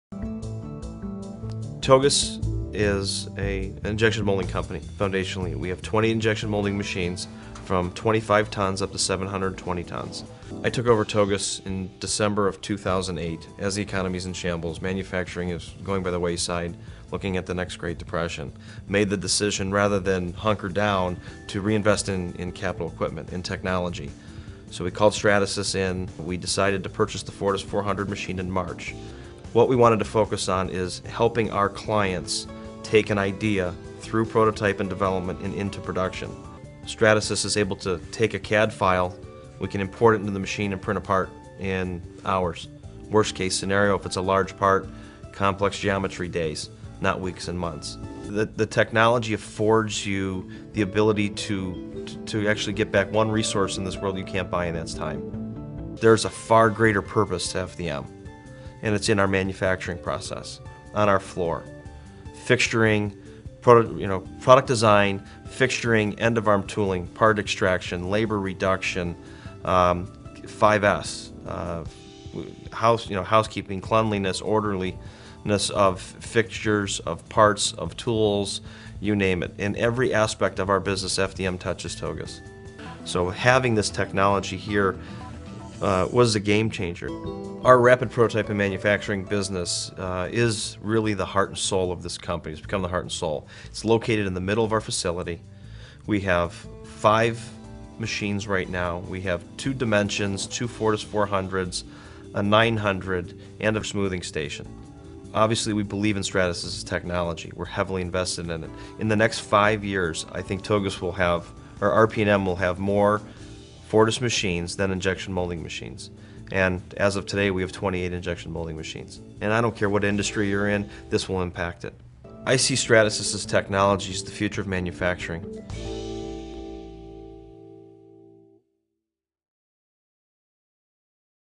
Thogus Chooses Stratasys and FDM Technology to Re Invent Itself
注塑成型公司利用 3D 列印自我革新